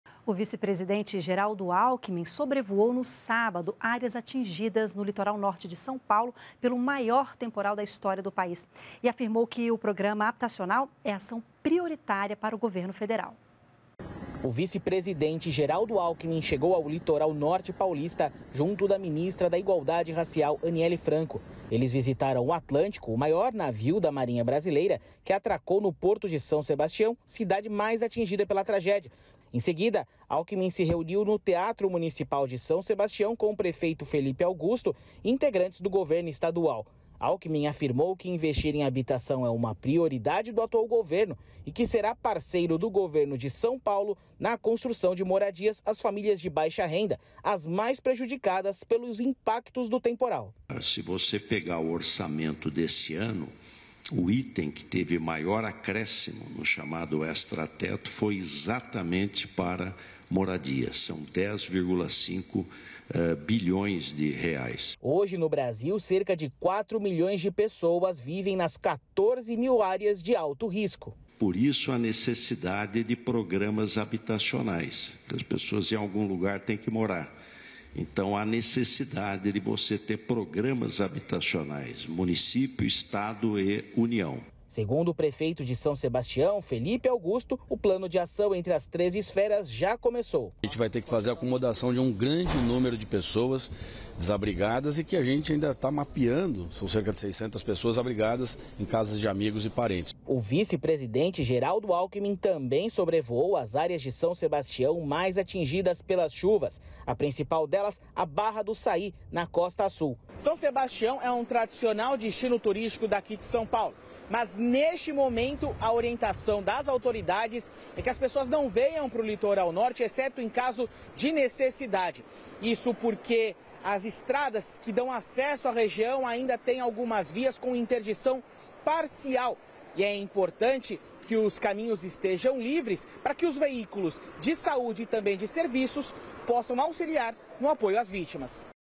Política | Litoral Norte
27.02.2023 - O vice-presidente Geraldo Alckmin sobrevoou, no sábado, áreas atingidas no litoral norte de São Paulo pelo maior temporal da história do país.